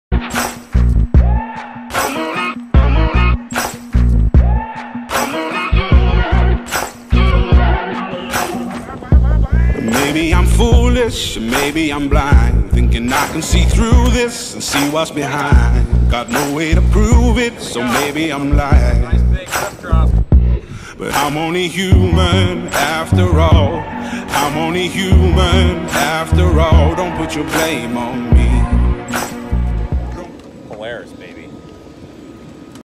Iraq Snowmobiling Video
Fun edit of me and my buddy who are missing the snow back home!